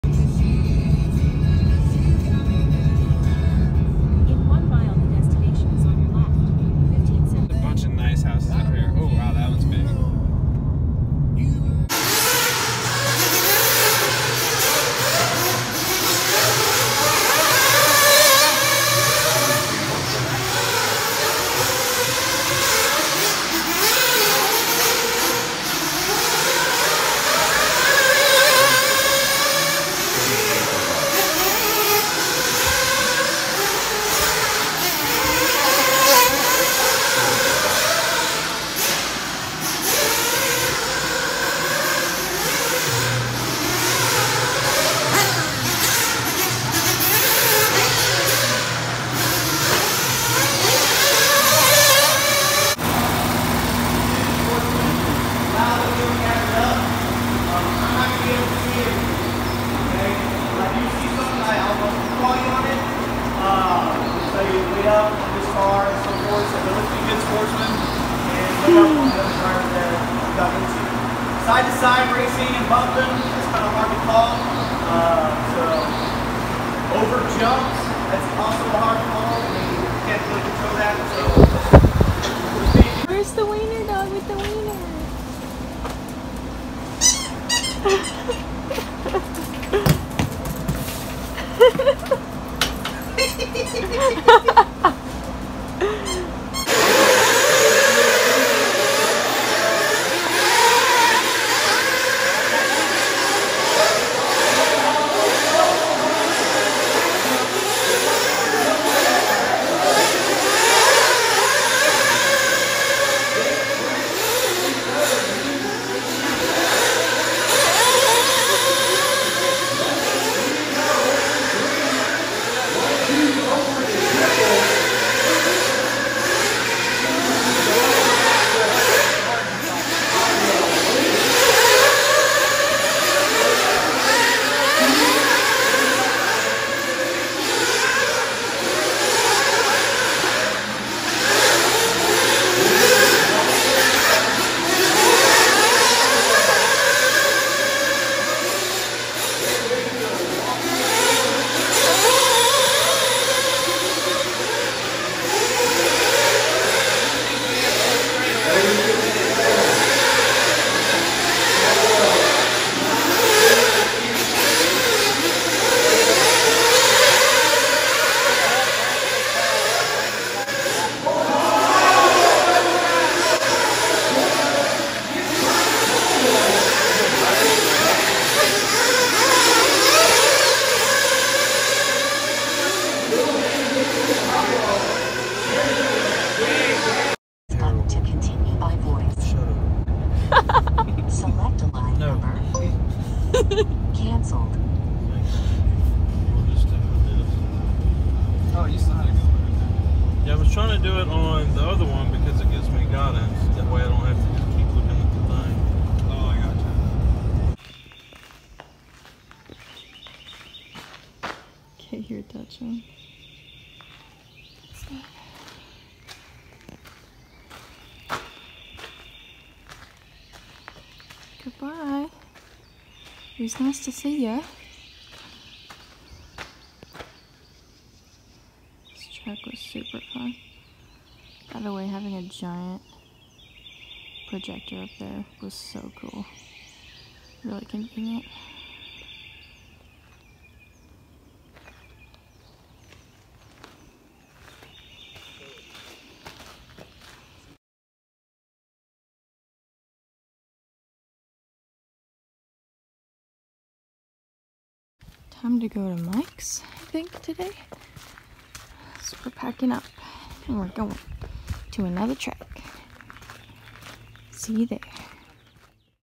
Texas Day 1 Track 1 - Thornhill Racing Circuit
Over the next 5 weeks, you will join us in reliving the memories from our RC Racing journey, track to track and state to state.